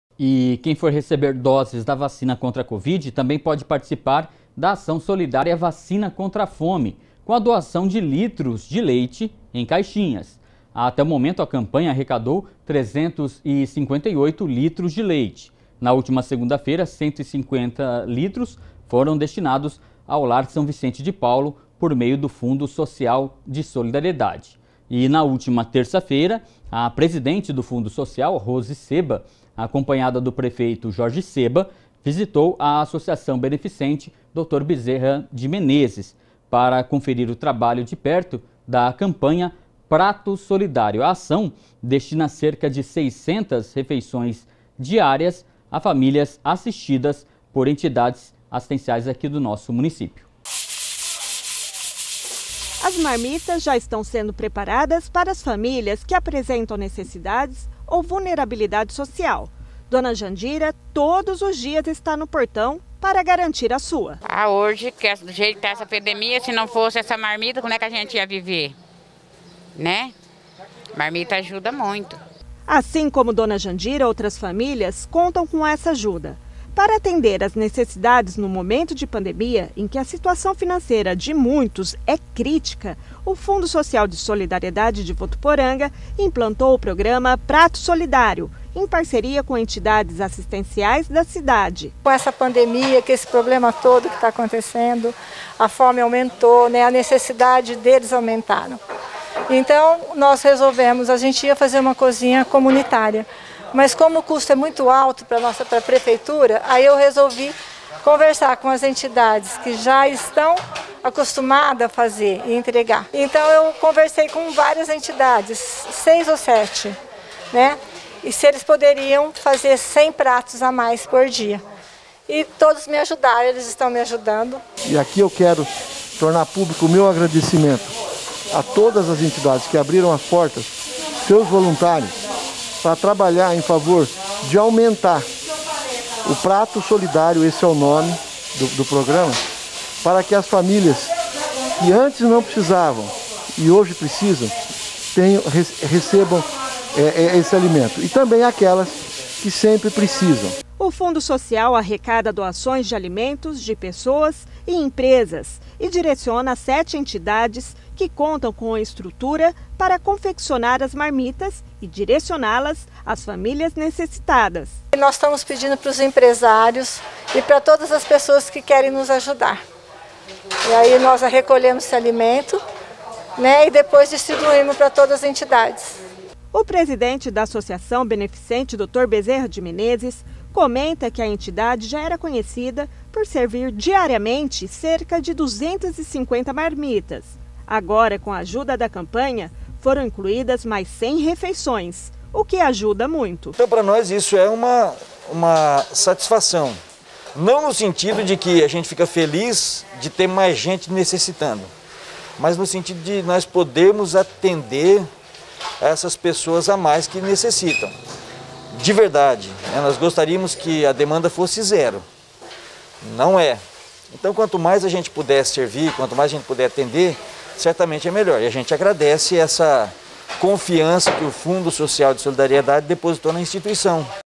Prato Solidário
A presidente do Fundo Social, Rose Seba visitou a Associação Beneficente Dr. Bezerra de Menezes para conferir o trabalho de perto da campanha prato solidário.  A ação destina cerca de 600 refeições diárias à famílias assistidas por entidades assitenciais do município.